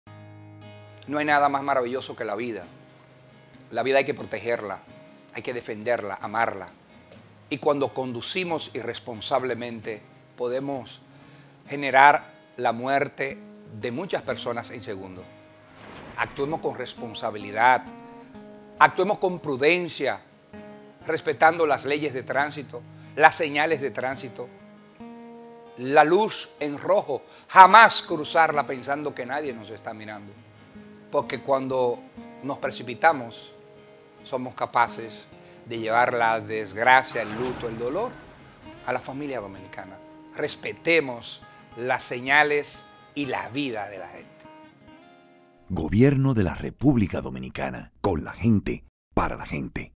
Julio César Valentín ‪EducacionSeguridadVialRD
‪Julio César Valentin, senador por Santiago: "Cuando nos precipitamos podemos llevar luto a familias dominicanas" ‬